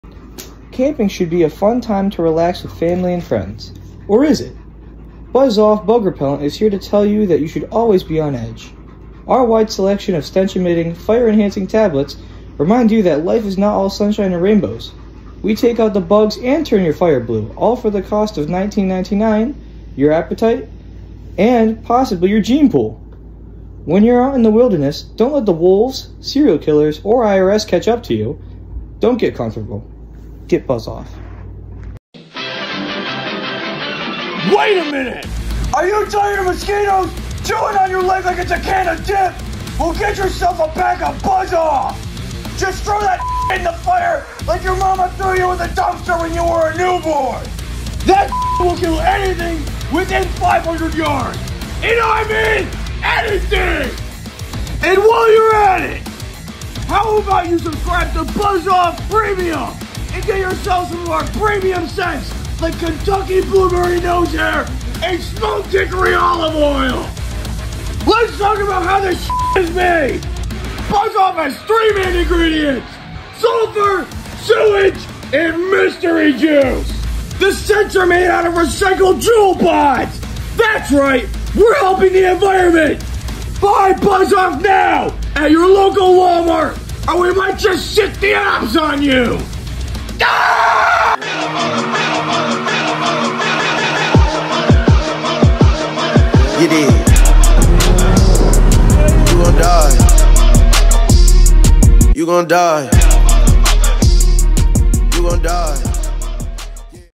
BUZZ-OFF!!!
BUZZ-OFF is our new line of bug repellent that you just throw in the fire and let it do its job! IT REALLY STINKS. But now at your local Walmart :)

This is a school project.